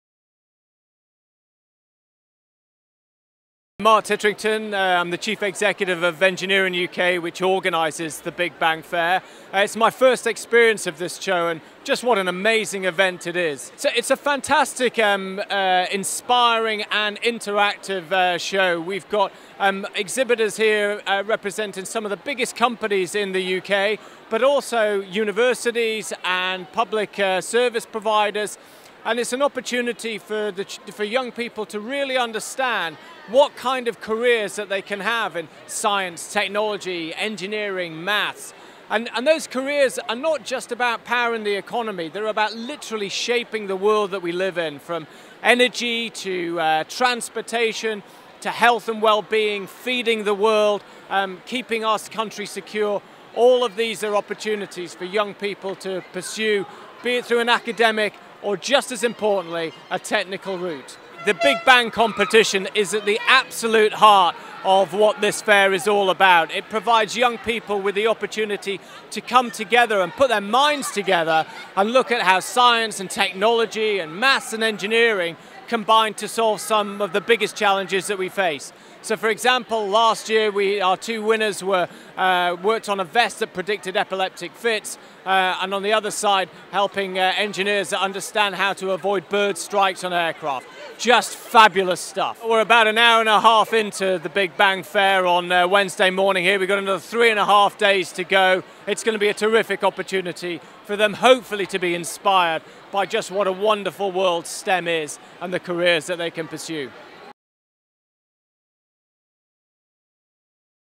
Mark Titterington introduces The Big Bang Fair 2018
Mark Titterington, Chief Executive of EngineeringUK introduces The Big Bang Fair 2018